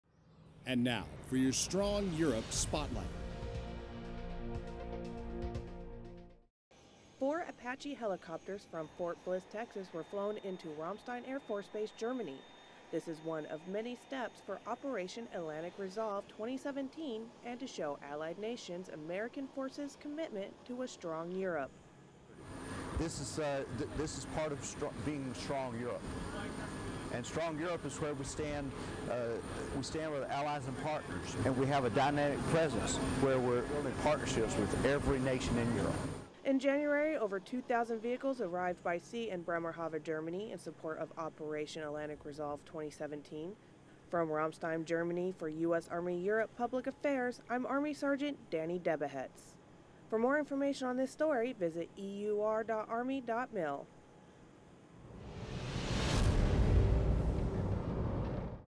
Strong Europe Spotlight 10 CAB
4 Apache Helicopters were flown in on 2 C-5 Galaxy planes from Fort Bliss, Texas to Ramstein Air Force Base, Germany. They will be in Europe for 9 months during Operation Atlantic Resolve 2017.